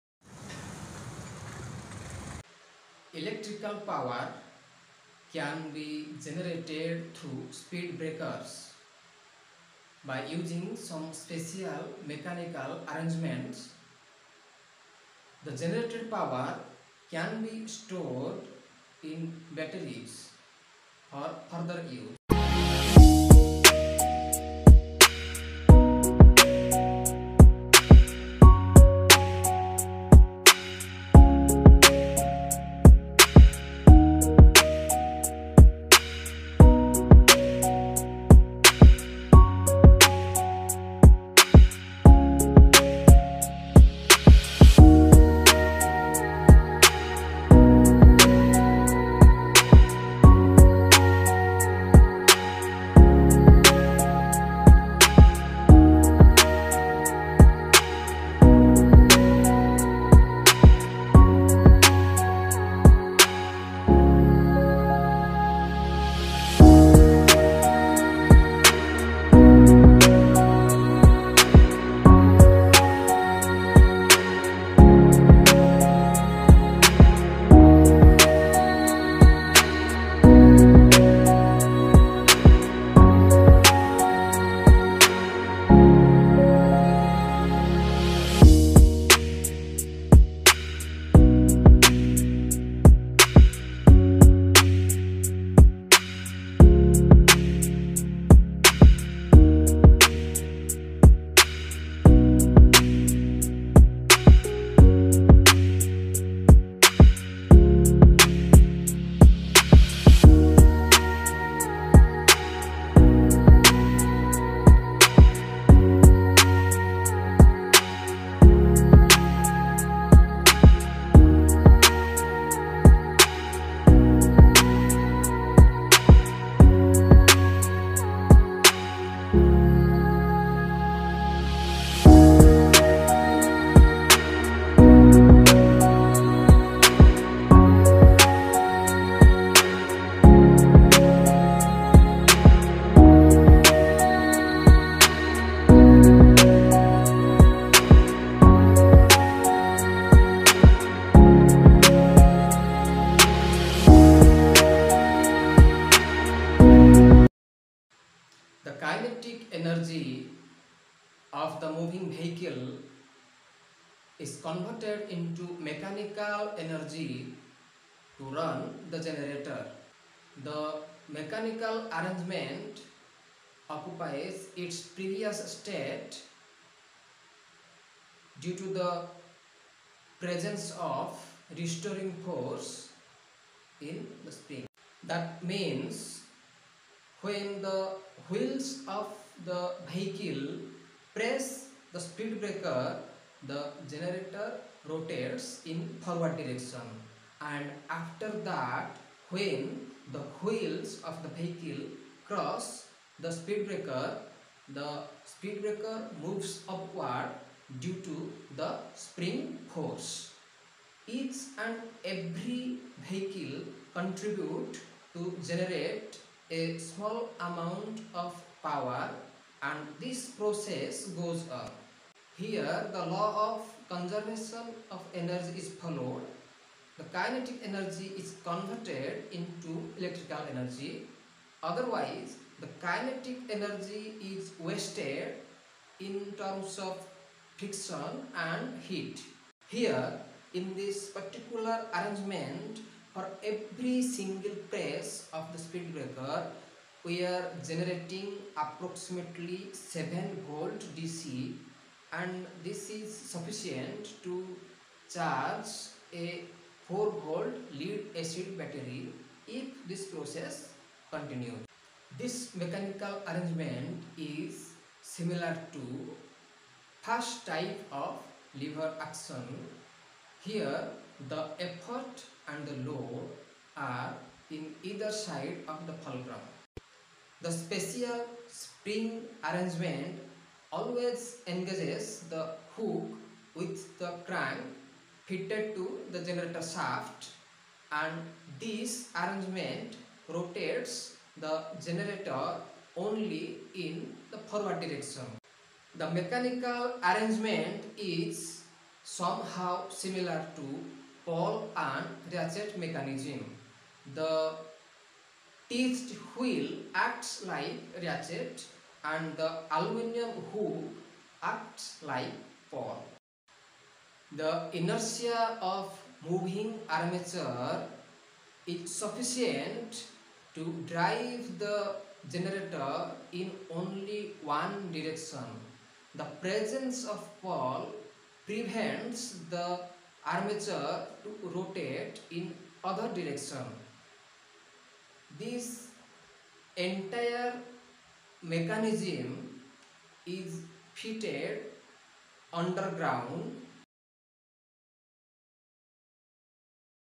ELECTRICAL POWER GENERATION THROUGH SPEED BREAKER
This video clearly shows you how to prepare a speed breaker from garbage. By using a PMDC motor , pawl and ratchet mechanism Electrical power can be generated through speed breaker.
This obeys Law of conservation of energy principle.
This support first type of lever action.
It is just like harvesting free electricity ELECTRICAL POWER GENERATION THROUGH SPEED BREAKER.

Pawl and Ratchet mechanism.

Law of conservation of Energy.

Conversion of Kinetic energy into Electrical energy.